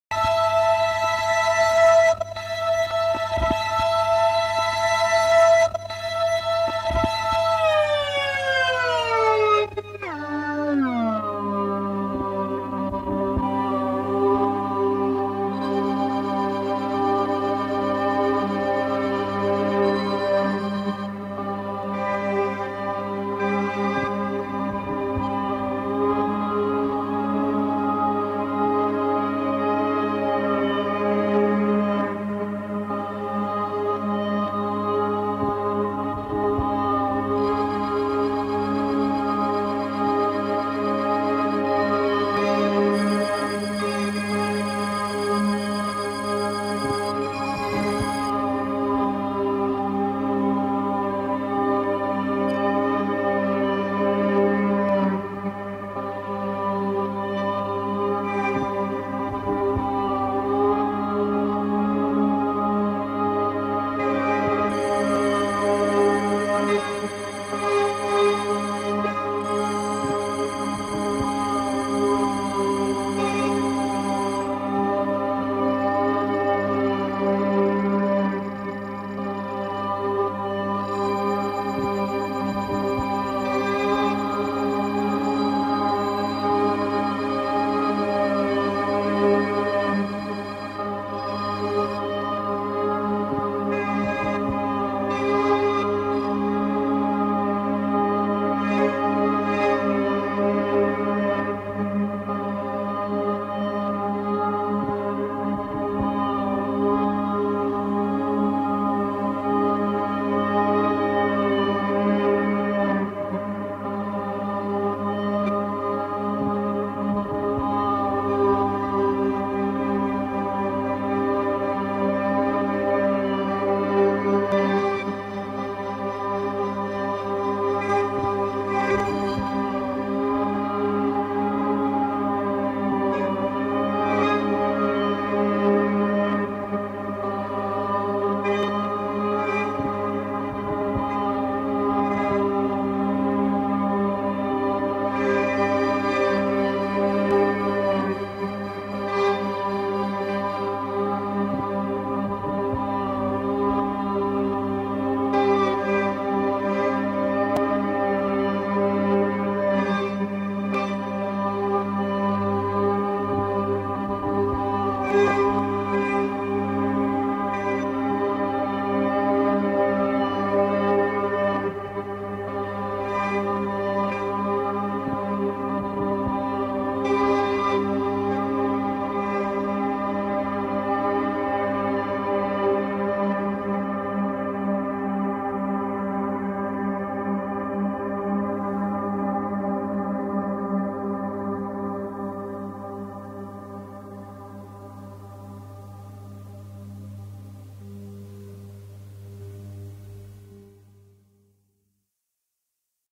Hyperspeed recording tapeloop turns into a scary drone
high sounding drone is pitched down many times over.  Recorded at hyper speed.  With some reverb and tape wobbling, it becomes a disturbing tapeloop pad